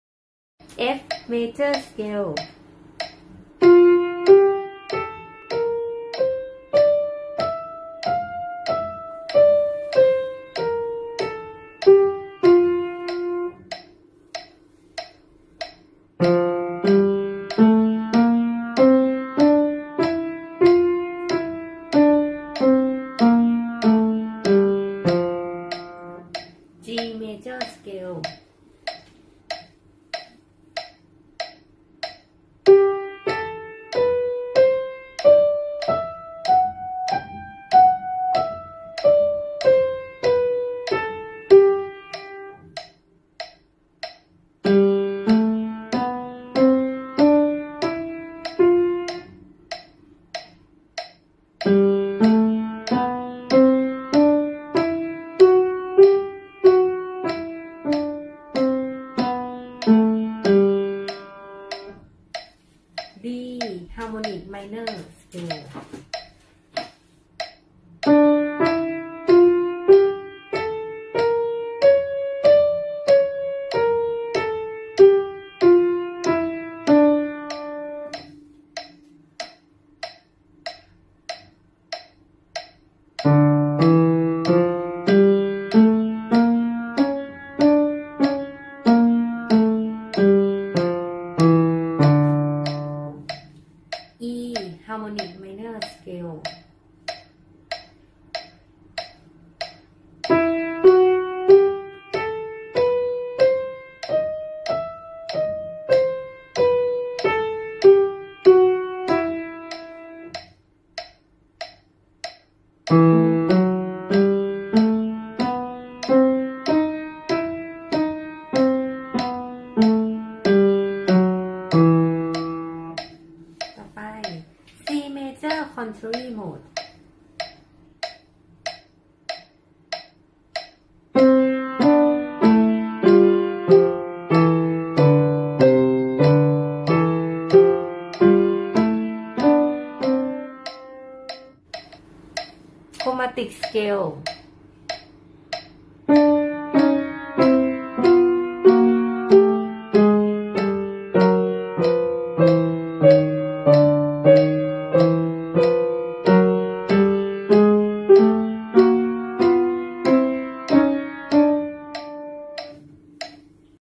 Namo Piano​ Grade​ 1
Scales
▪️​F  Major
▪️​G Major
▪️​D Harmonic​ Minor
▪️E Harmonic​is Minor
▪️ C Major​ Contrary Motion
▪️ Chromatic Scales